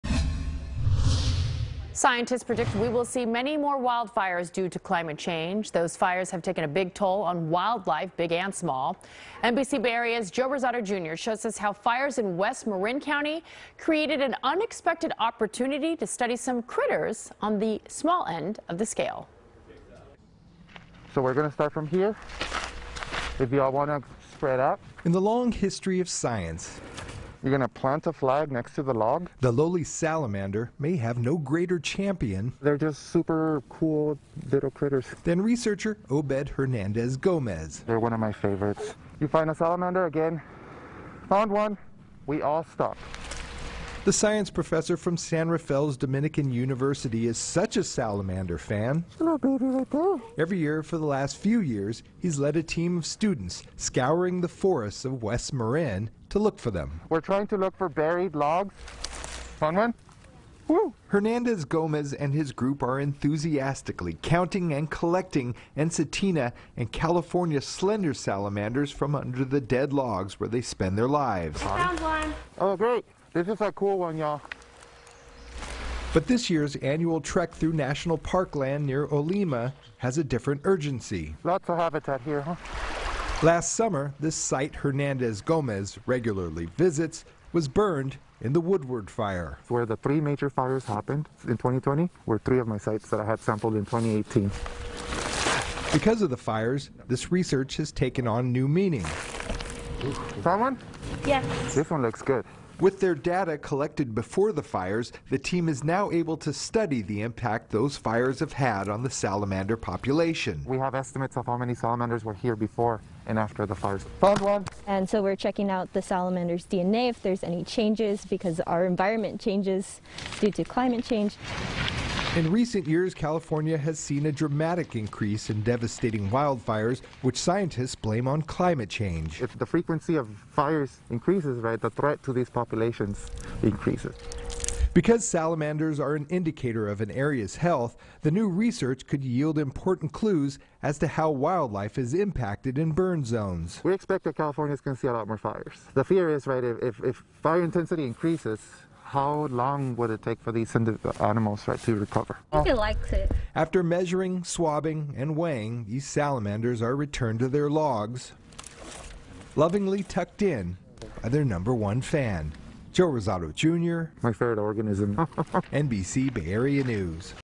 Local Salamander Study Could Shed Light on Life in Fire Zones
With California now facing yearly threats of devastating wildfires, a Dominican University researcher is looking to salamanders as a potential indicator of the fires’ impact on wildlife. Joe Rosato Jr. reports.